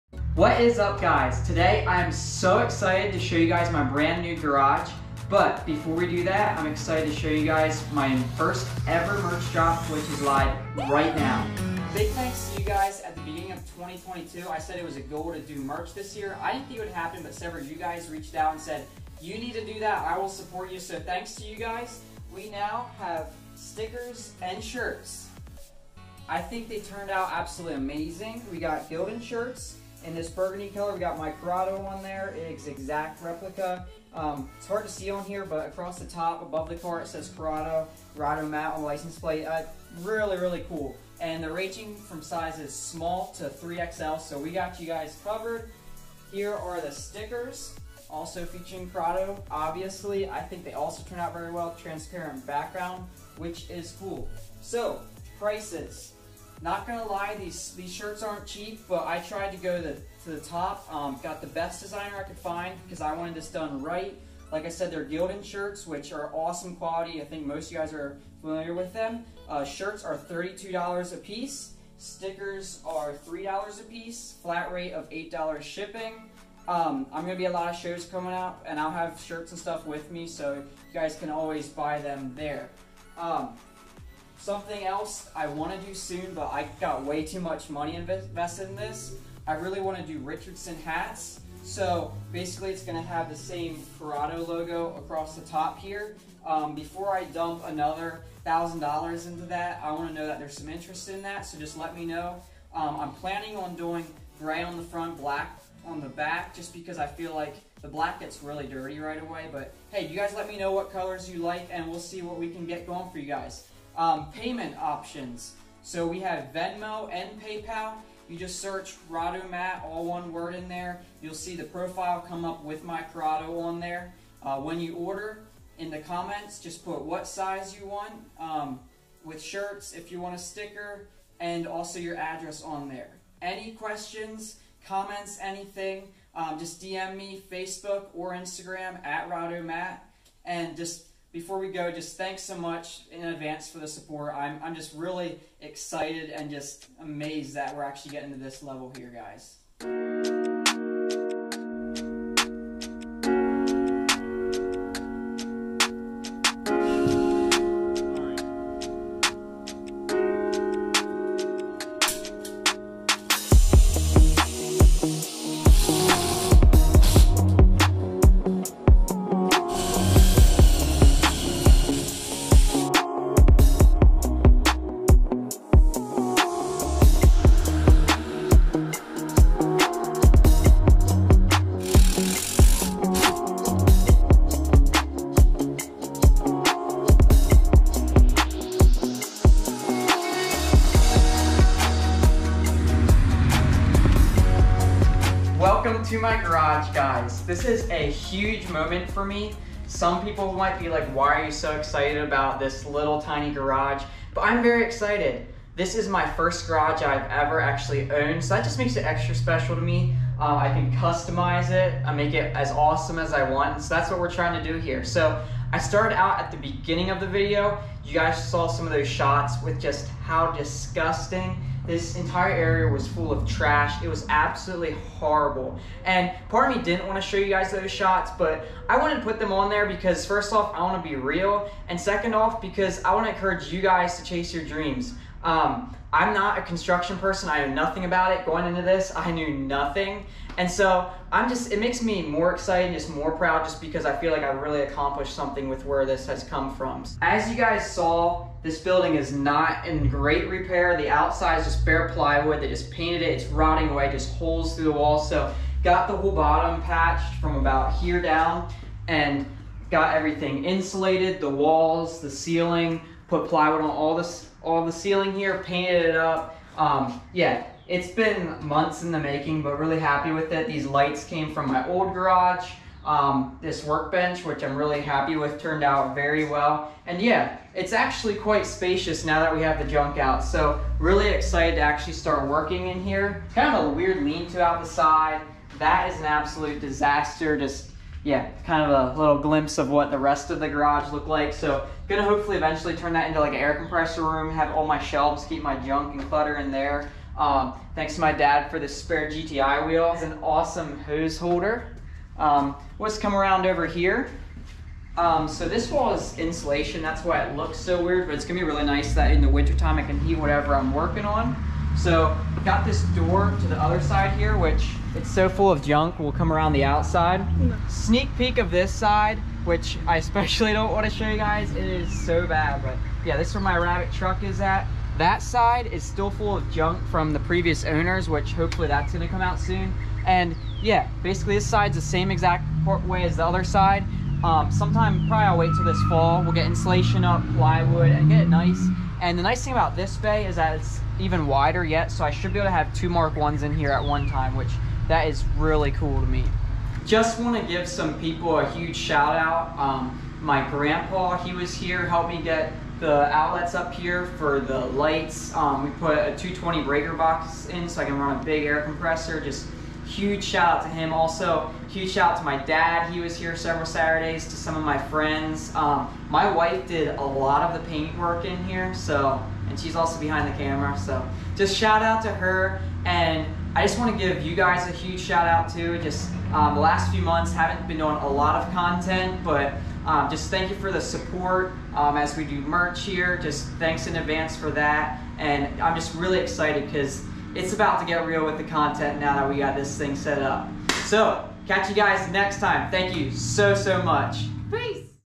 Corrado Merch Drop, and New Garage Tour!
This video has been months in the making! So excited to reveal my first ever merch drop and my new garage!!

Note - I am willing to send merch out of America, I will just charge exactly whatever shipping costs me.